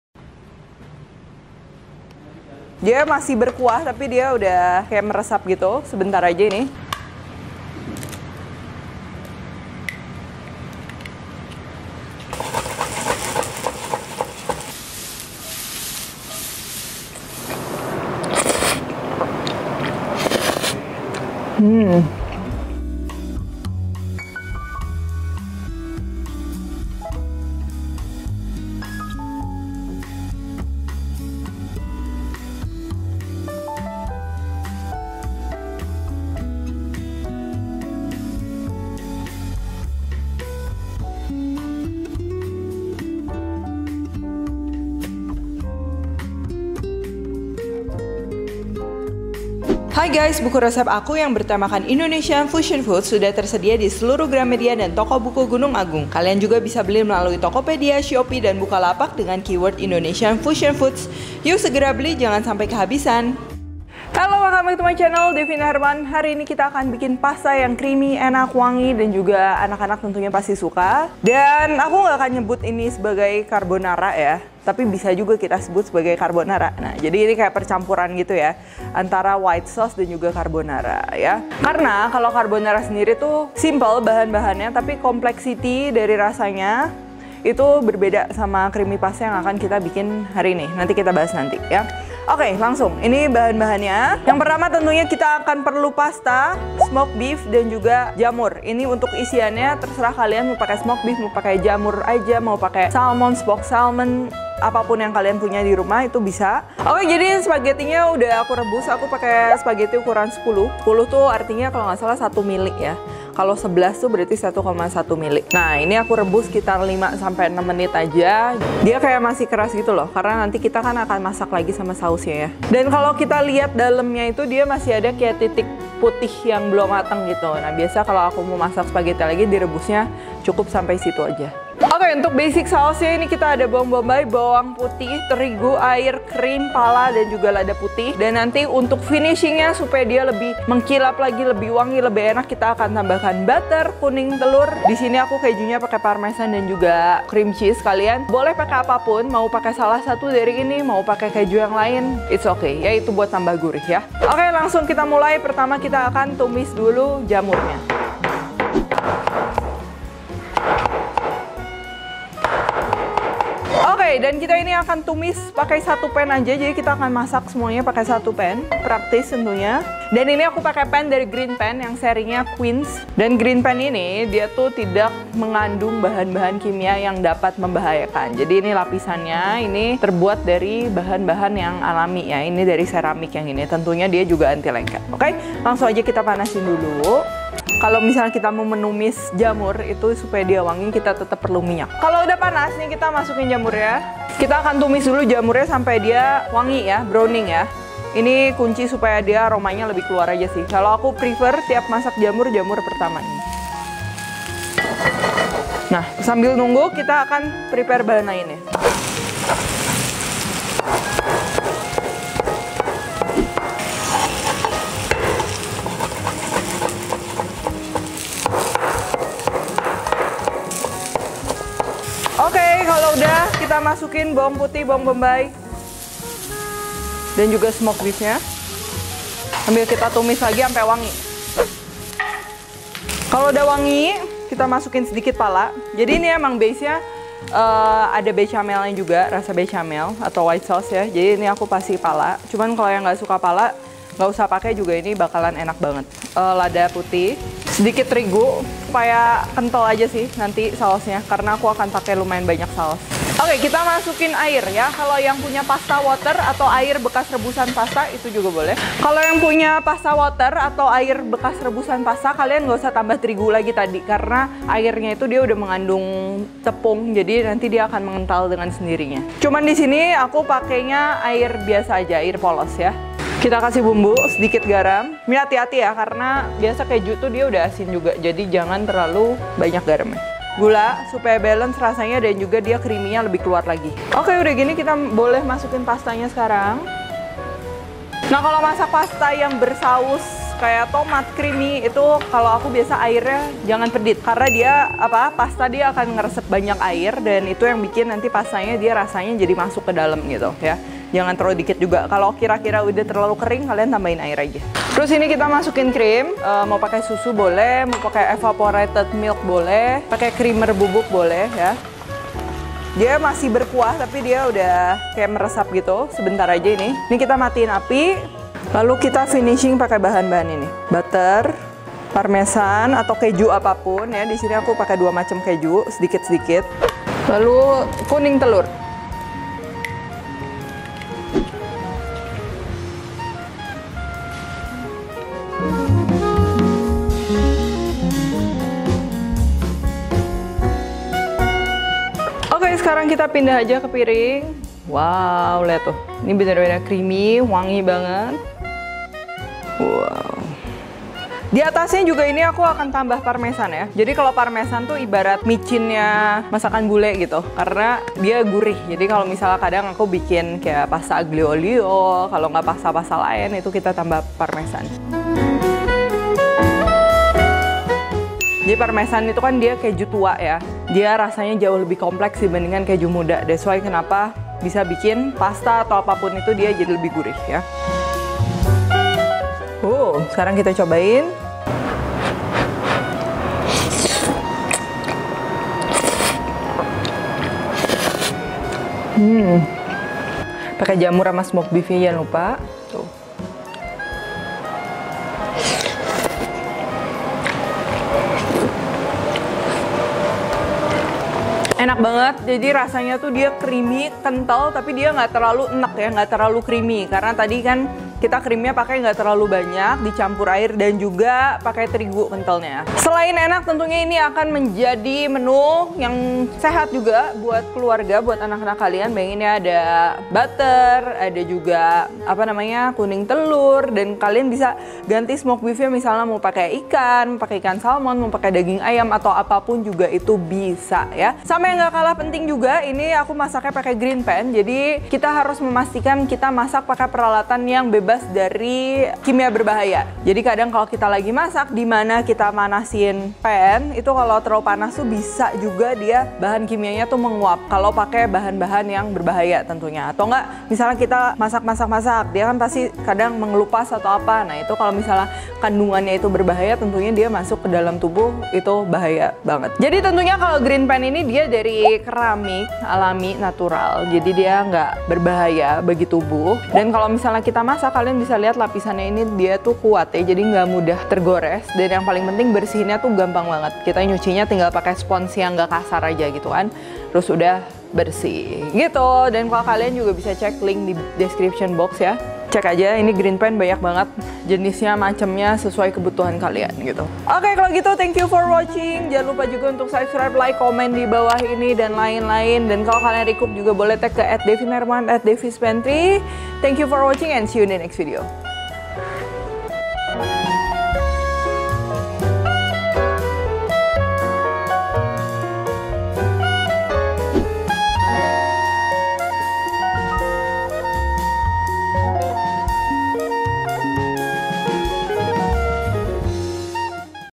Greenpan - Pasta Creamy Carbonara with Devina Hermawan
Resep Creamy Pasta. Banyak sekali sekali variasi dari masakan Italia ini, salah satunya creamy pasta. Kali ini aku akan share resep creamy mushroom spaghetti favorit keluarga. Pasta dengan saus creamy dari campuran susu, keju, dan kocokan telur ini memiliki rasa gurih manis, cocok untuk anak-anak.

Resep Creamy Pasta (untuk 2 porsi)

Bahan:
120 gr spaghetti ukuran 10
10 jamur champignon, iris
2 lembar smoked beef, iris

Creamy sauce:
¼ siung bawang bombai, cincang
3 siung bawang putih, cincang
¼ sdt pala
½ sdt merica
½ sdm tepung terigu
100-200 ml air
80 ml cream
1 sdm mentega
1 sdm parmesan
1 sdm cheese spread
1 butir kuning telur
½ sdt garam
1 sdt gula

Langkah:
1. Rebus spaghetti ¾ matang ±5-6 menit, tiriskan
2. Panaskan pan, tumis jamur  dengan sedikit minyak hingga kering lalu tambahkan bawang bombai, bawang putih, dan smoked beef lalu tumis lagi hingga wangi
3. Tambahkan merica, pala bubuk, dan tepung kemudian masak hingga wangi
4. Masukkan air dan spaghetti kemudian bumbui dengan garam dan gula, masak hingga air sedikit surut lalu tambahkan cream, masak hingga meresap
5. Tambahkan mentega, parmesan, dan cheese spread aduk rata dan matikan api
6. Tambahkan kuning telur, aduk rata
7. Sajikan Creamy Pasta dengan garnish keju parmesan